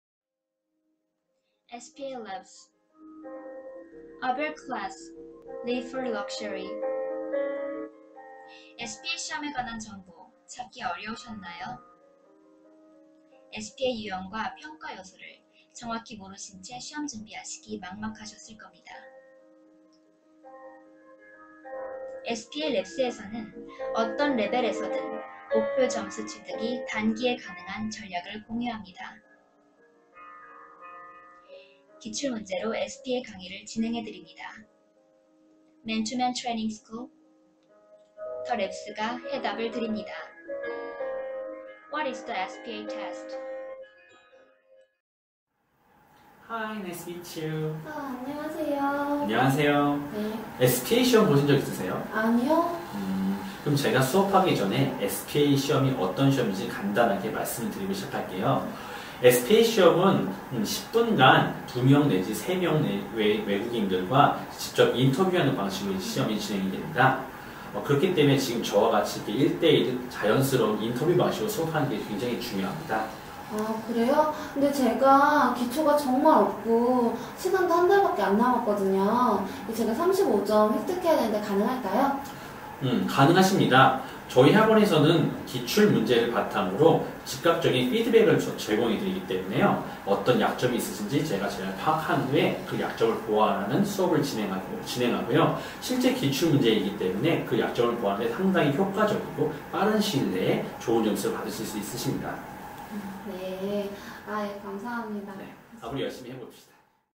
[SPA LABS] SPA 시험 전략 독점 공개
SPA 시험 유형 및 SPA 공부 방법 소개 동영상입니다. 실제 SPA LABS 김태희 강사가 1:1로 강의 진행하는 모습을 담았습니다. 동영상과 관련된 자세한 문의는 / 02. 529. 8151 / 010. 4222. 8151 으로 문의 주시면 자세히 설명드리겠습니다.